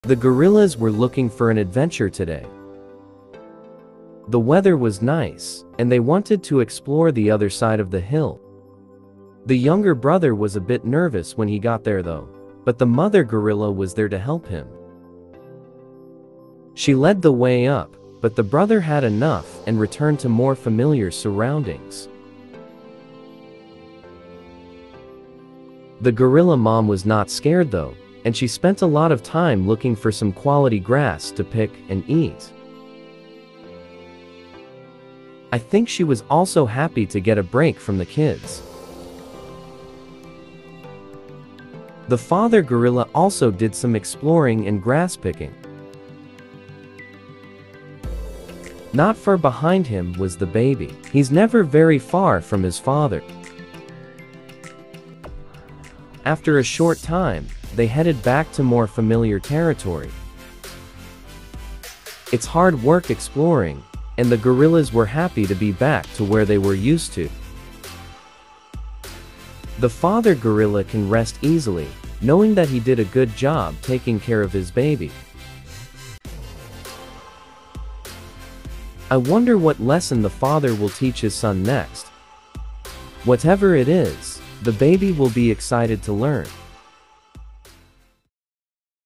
Gorillas do Some Exploring!
The gorilla's got a bit more curious today and went over to the other side of the hill to explore. Let's watch and see what they find!